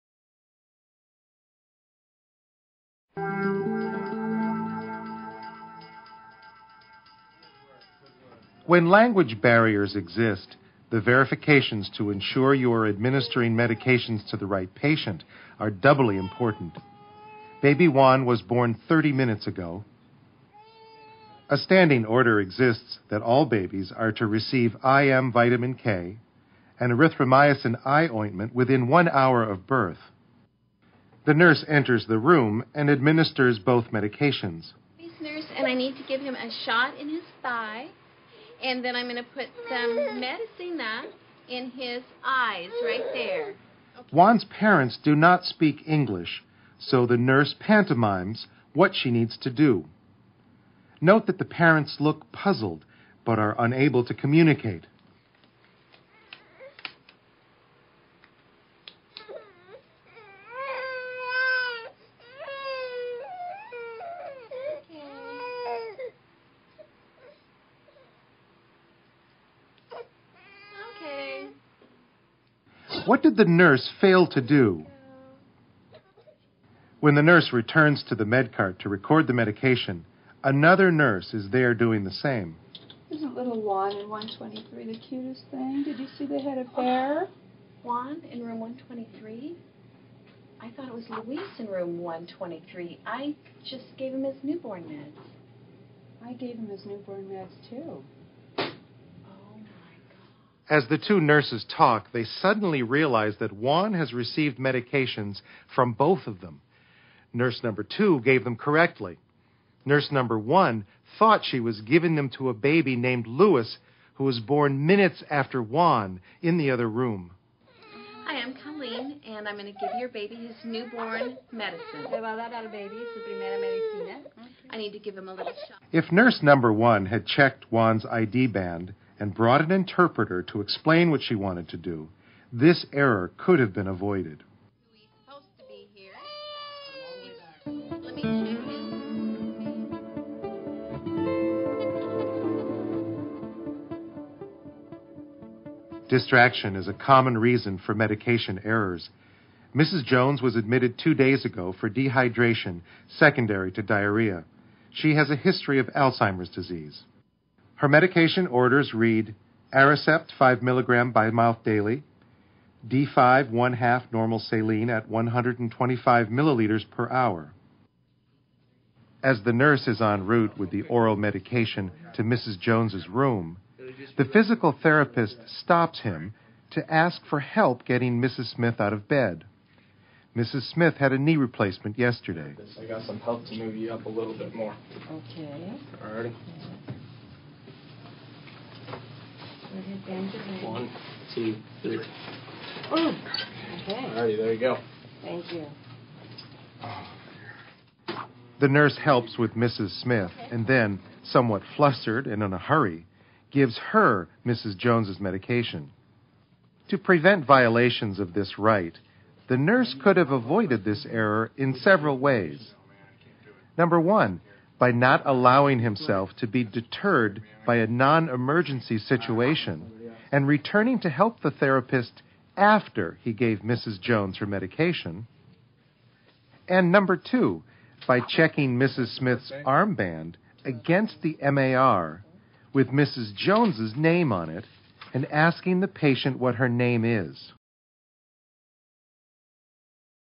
Error Prevention
1. Describe in detail each of the Six Rights of Medication Administration.
2. State the components of a complete medication order.
3. Identify the medication errors made in the case studies presented.
4. Discuss methods in which the errors made in these scenarios could have been prevented.

Product ID: CLP075
Year: 2011

Other Programs Included in this Series:
The Six Rights